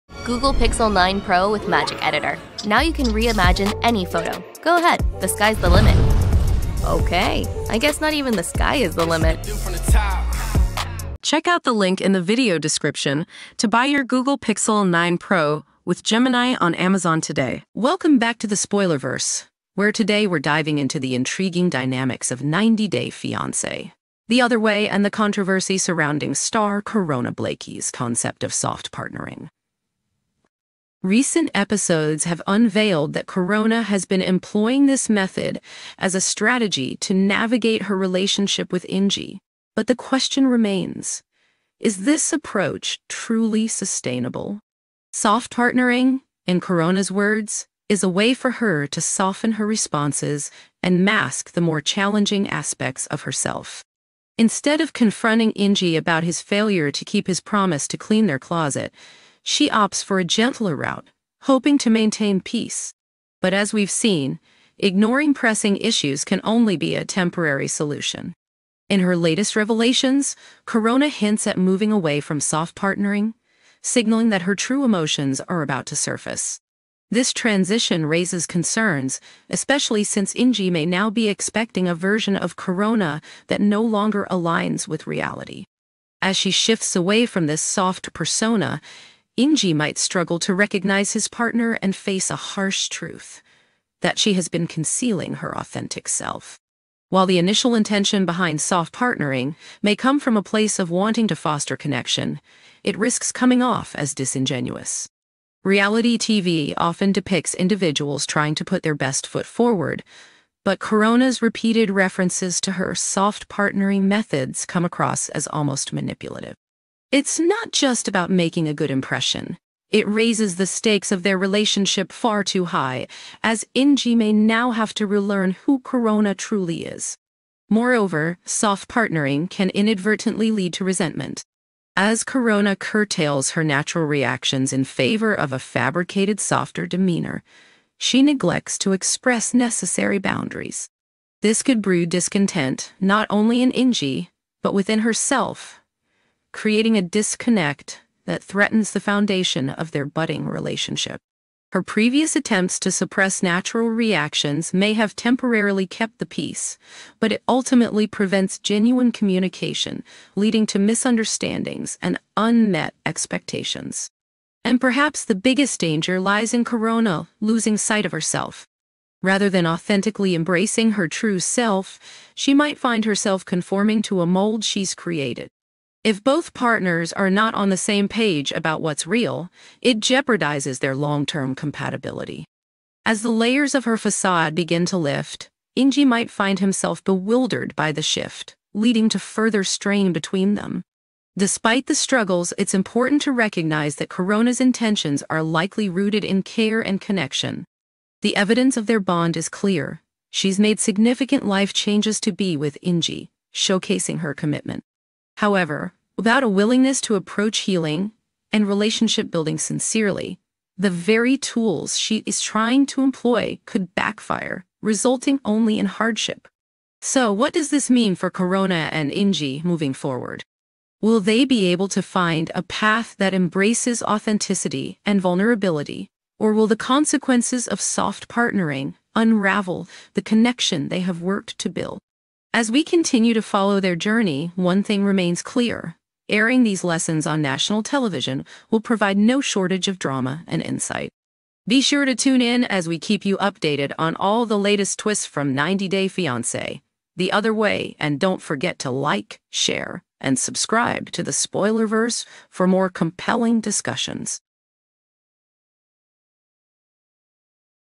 Unmasking 'Soft-Partnering': Is Corona's Strategy Sabotaging Her Relationship on 90 Day Fiancé?
Buy your Google Pixel 9 Pro with Gemini on Amazon today
Join us in this deep dive into the complex relationship dynamics on '90 Day Fiancé: The Other Way' as we explore Corona Blakey's controversial approach to 'soft-partnering.' Is this technique a helpful way to navigate her relationship with Ingi, or is it masking deeper issues that could threaten their connection? We'll examine how Corona's desire to present a softer version of herself may lead to misunderstandings, resentment, and a loss of authenticity. Tune in as we unpack the potential pitfalls of soft-partnering, the implications for their future, and what this means for their evolving relationship.